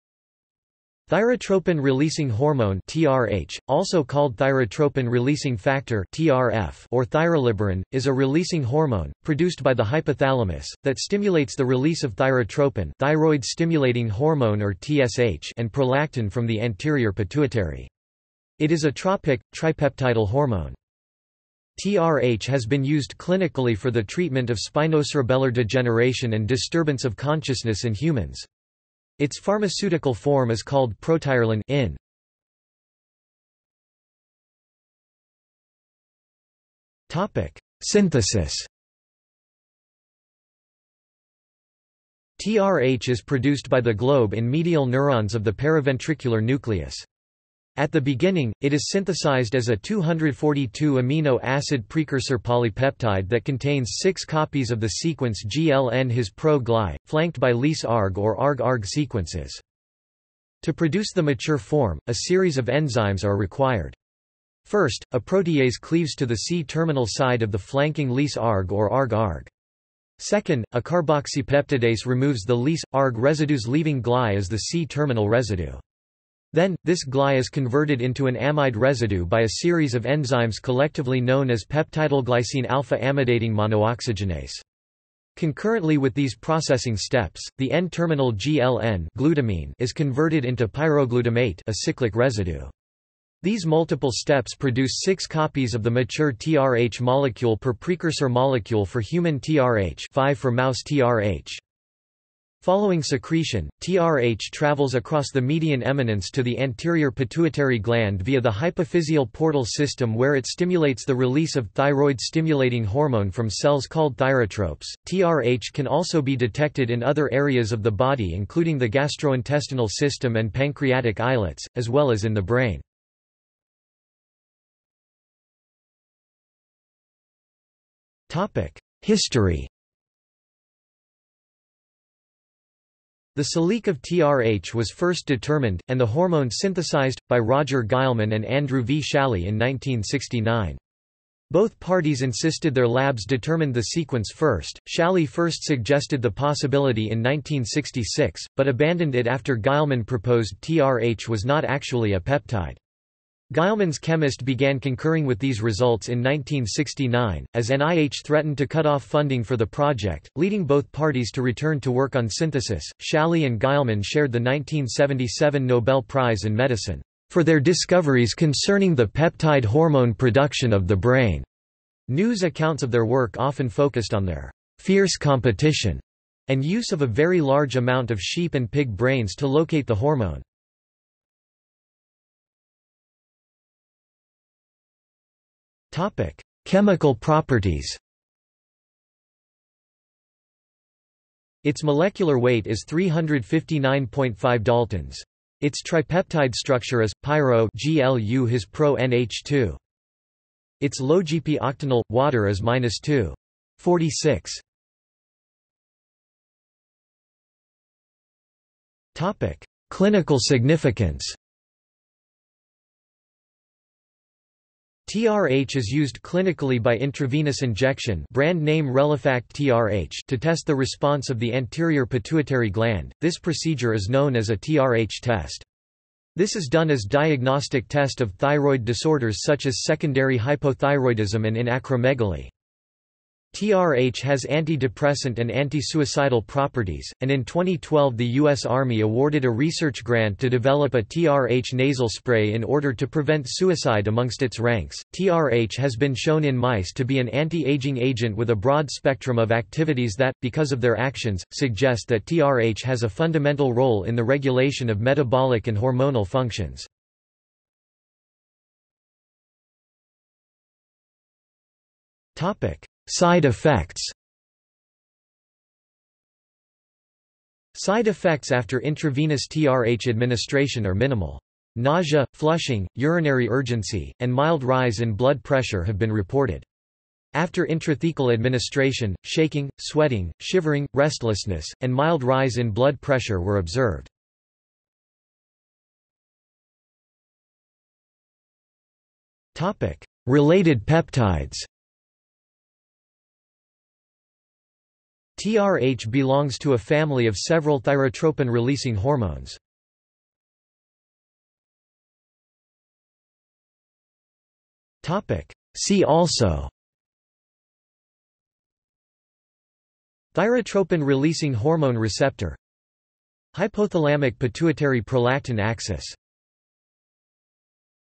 Protirelin | Wikipedia audio article
This is an audio version of the Wikipedia Article:
Protirelin

00:00:33 1 Synthesis
00:02:04 2 History
00:03:02 3 Chemical properties
00:03:25 4 Clinical significance
00:04:18 5 Side effects
00:04:45 6 Related peptides
00:05:01 7 See also

- Socrates

SUMMARY
Thyrotropin-releasing hormone (TRH), also called thyrotropin-releasing factor (TRF) or thyroliberin, is a releasing hormone, produced by the hypothalamus, that stimulates the release of thyrotropin (thyroid-stimulating hormone or TSH) and prolactin from the anterior pituitary. It is a tropic, tripeptidal hormone.
TRH has been used clinically for the treatment of spinocerebellar degeneration and disturbance of consciousness in humans. Its pharmaceutical form is called protirelin (INN) ().